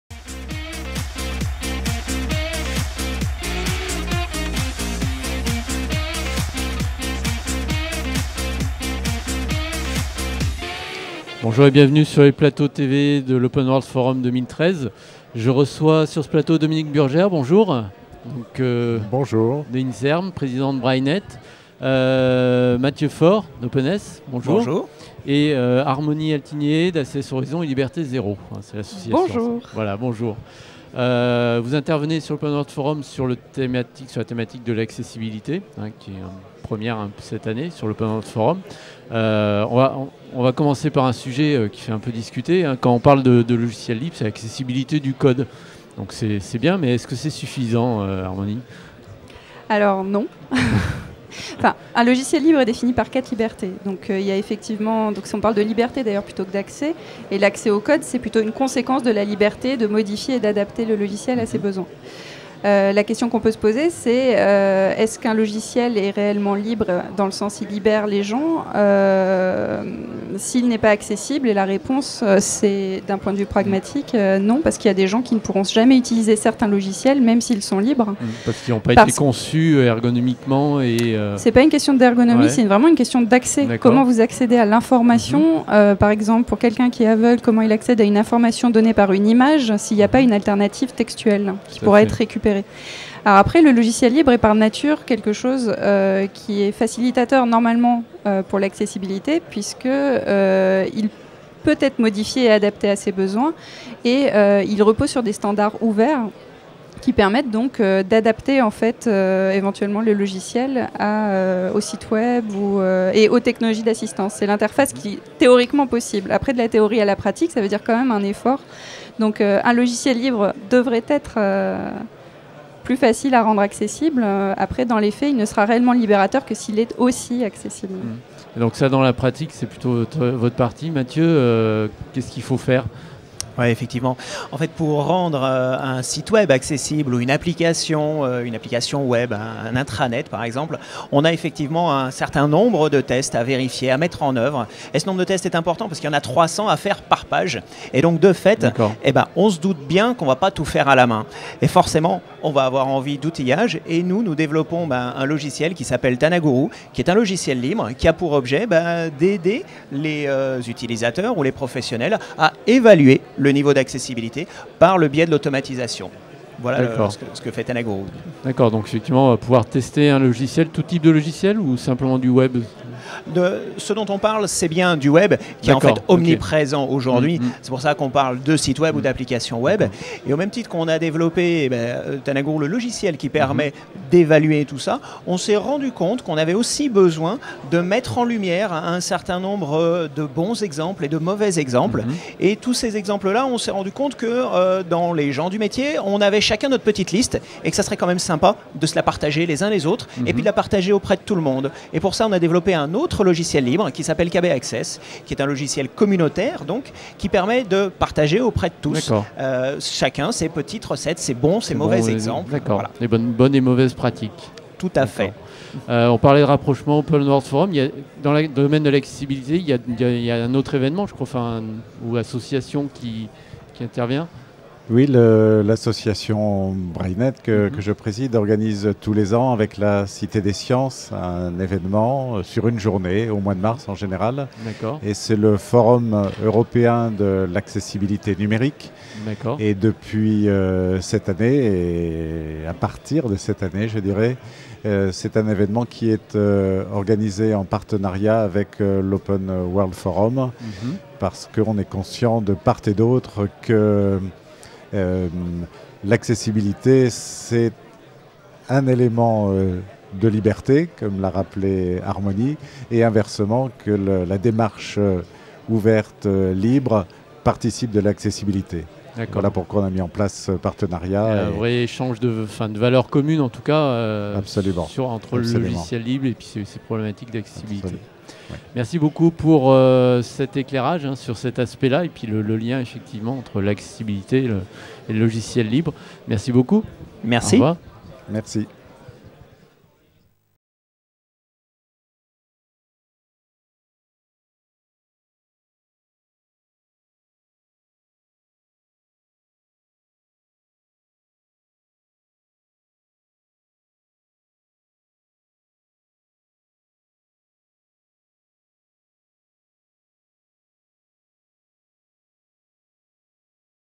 Accessibilité & Open Source
Interview d'Armony Altinier ( ACS Horizons Ltd / Liberté Zéro), Matthieu Faure ( Open-S ) et de Dominique Burger ( BrailleNet - INSERM )